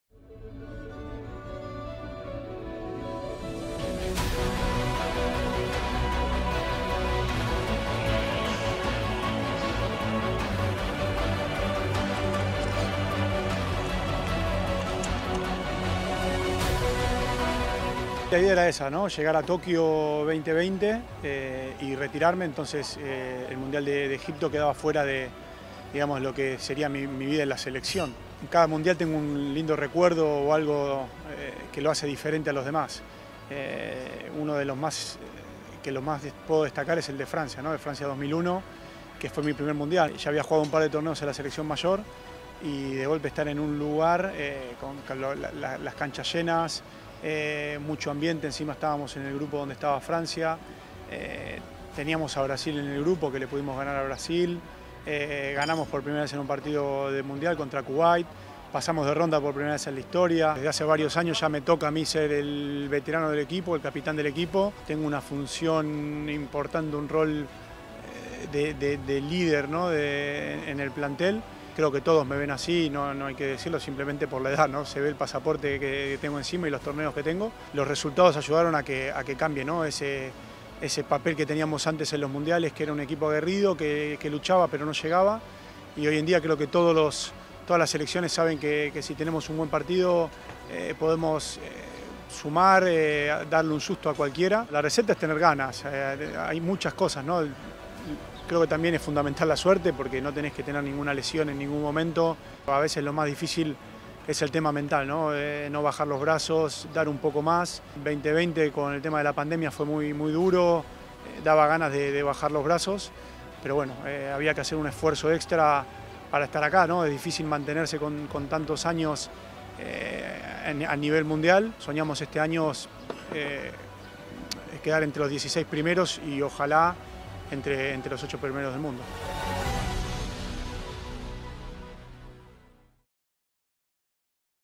Gonzalo Carou and his 11th World Championship
No other athlete at Egypt2021 has played as many World Championships as Argentina's Gonzalo Carou 🇦🇷: this is his 11th edition! The 41-year old pivot, captain of LosGladiadores, admits in this difficult year he thought of giving up but decided he had to do one more effort 👊